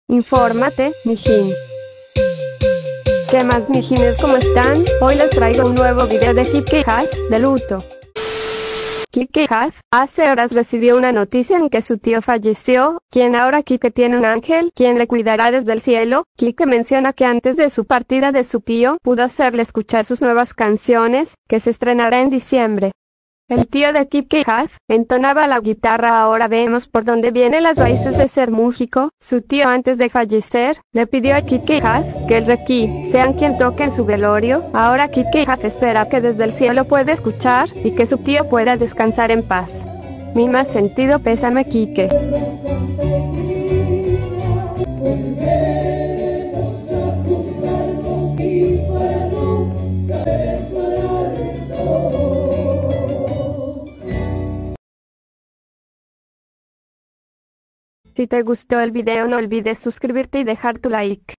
KIKE JAV DE LUTO
NFÓRMATE MIJIN!!

En el siguiente video veremos a KIKE JAV, haciendo planes para viajar al mundial.